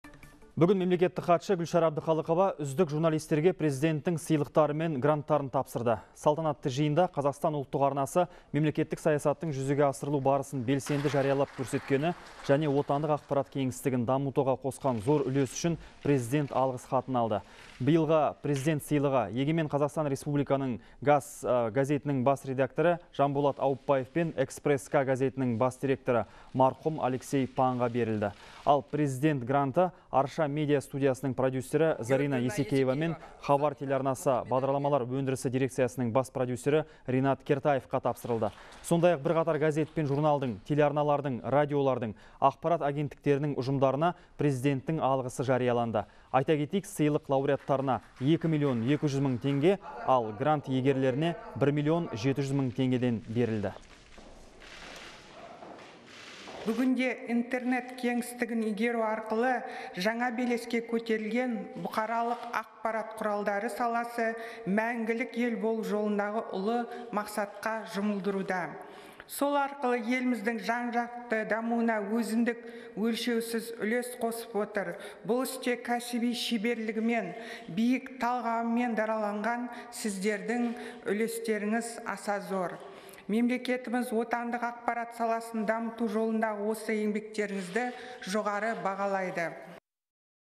Мемлекеттік хатшы Г.Әбдіқалықова үздік журналистерге Президенттің сыйлықтары мен гранттарын тапсырды
«Қазақстан» Ұлттық телеарнасы мемлекеттік саясаттың жүзеге асырылу барысын белсенді жариялап көрсеткені және отандық ақпарат кеңістігін дамытуға қосқан зор үлесі үшін Президент атынан Алғыс хат алды.